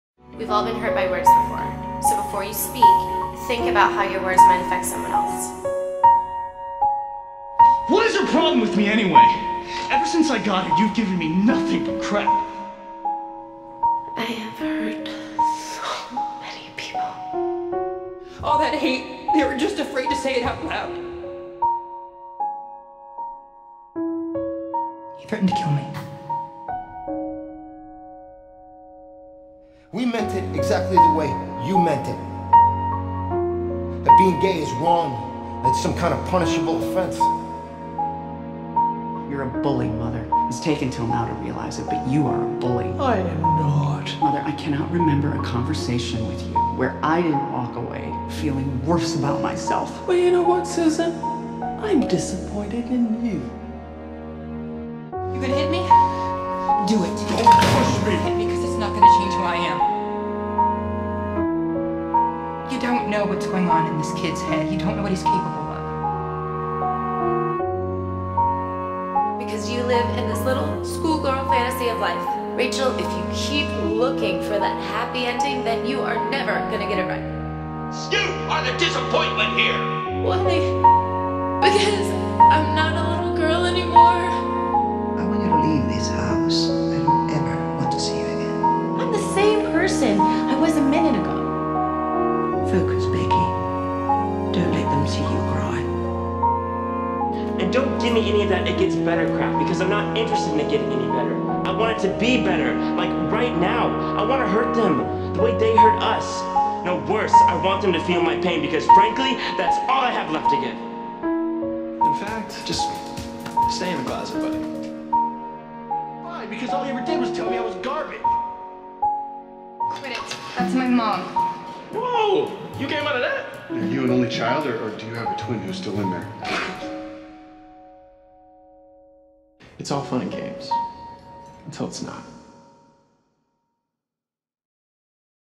● Cruelty of the words || eelG [TTB] ●
Entry about "Cruelty" for the first round of the "Time to Begin" contest. Reading the word "Cruelty" makes most people think about pain and torture of the body itself but I tried to focus on the cruelty, that words can create. Words can cause pain, they can be mean and cruel and they can lead to bad things. So yes, I might not have used the word as most of the people would have, but I hope I still got the contest rules right with my interpretation :)

{ Information }
► Coloring: Mine
► Footage: Please don't mention the name it's in the video

All music, clips, animations, overlays, textures, photos, etc. belong to all respective artists. This video was just made for fun and NO profit.